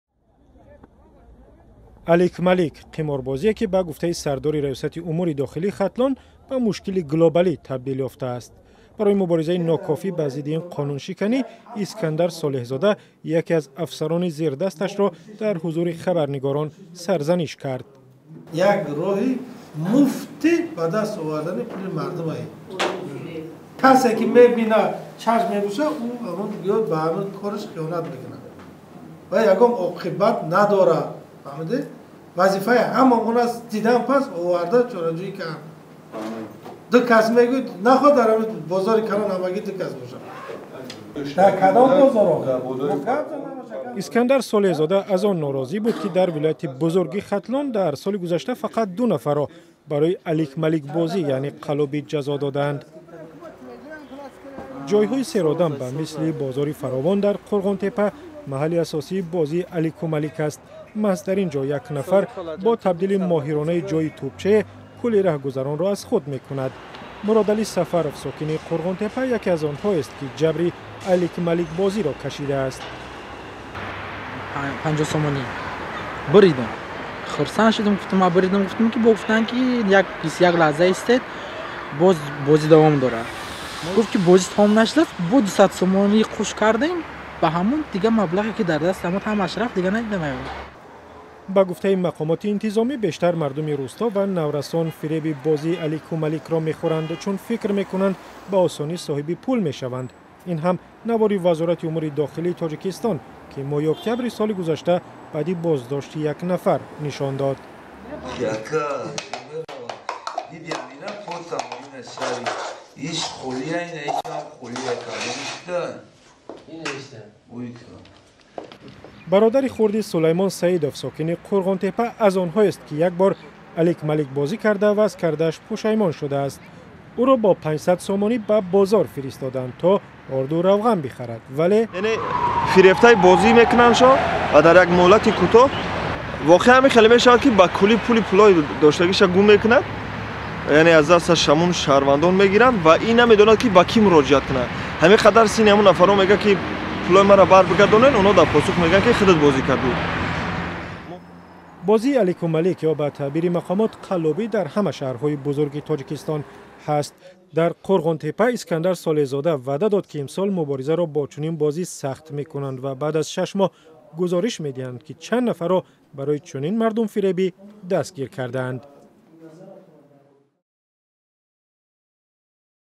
Корманди милиса барои алик-маликбозӣ гап шунид
Сардори раёсати умури дохилии Хатлон, афсарони милисаро танқид кард, ки ба зидди қиморбозӣ дар кӯчаҳои Қӯрғонтеппа дуруст мубориза намебаранд.